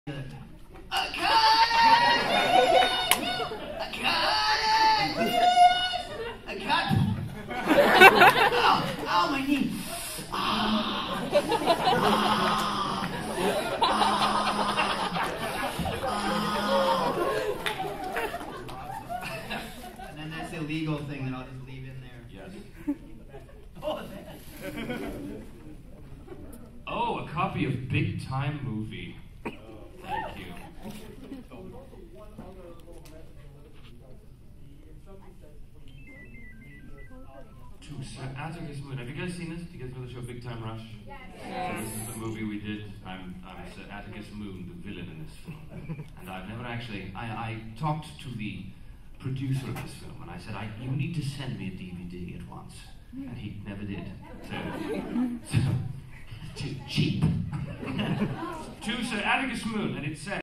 March 10, 2013 Toronto Comic Con Pt. 2: Richard Cox playing with dolls
This was so funny. Somebody had given him a gift bag, so he took the Kagome and Inuyasha dolls in it and did their little cliched name-calling back and forth.